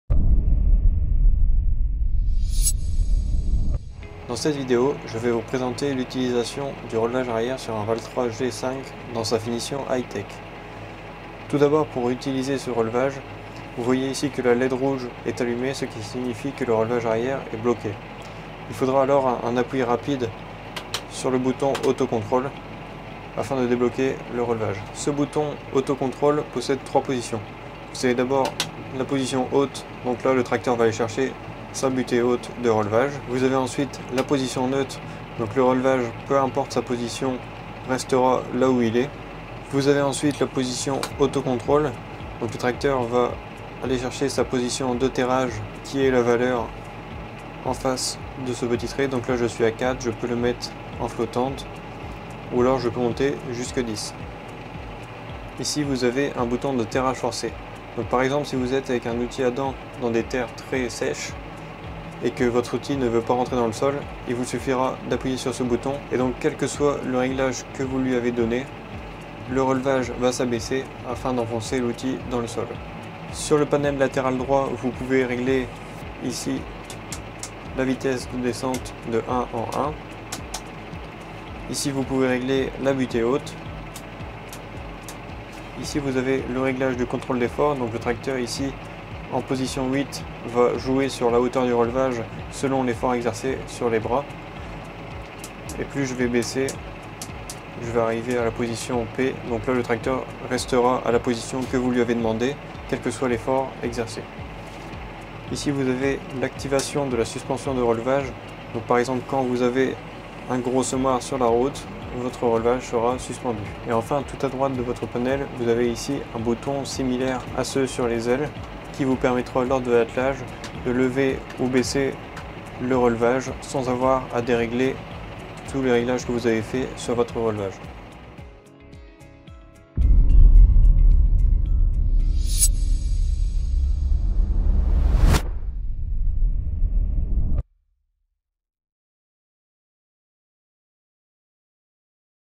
Comment utiliser le Relevage du Valtra G5 ?
Dans cette vidéo, nous vous présentons comment bien utiliser les différentes fonctions du relevage arrière sur un Valtra G5 en finition Hi-tech.